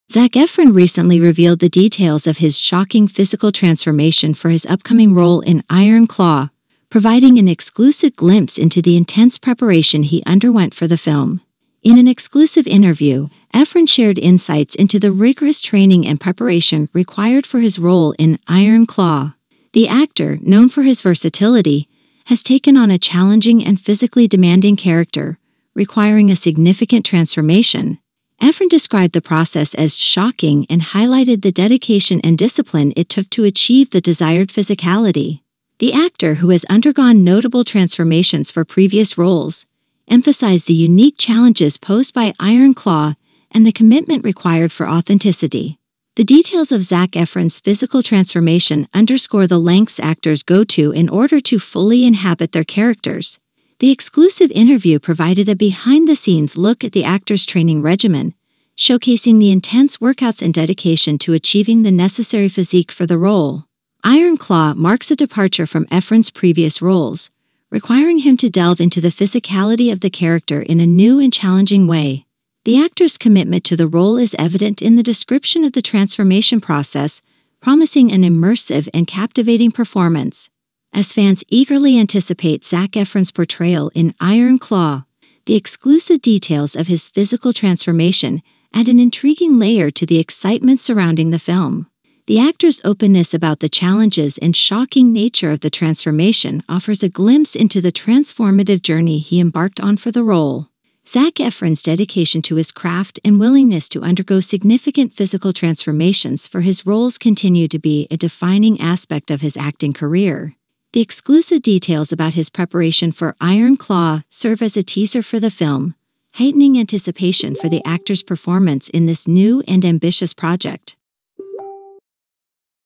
Zac Efron Details His 'Shocking' Physical Transformation For 'Iron Claw' Role Exclusive
Get an exclusive behind-the-scenes look at Zac Efron's jaw-dropping physical transformation for his upcoming role in 'Iron Claw.' The Hollywood heartthrob shares the intense training regimen, diet secrets, and the challenges he faced to embody the character. Don't miss this exclusive glimpse into the actor's dedication to his craft.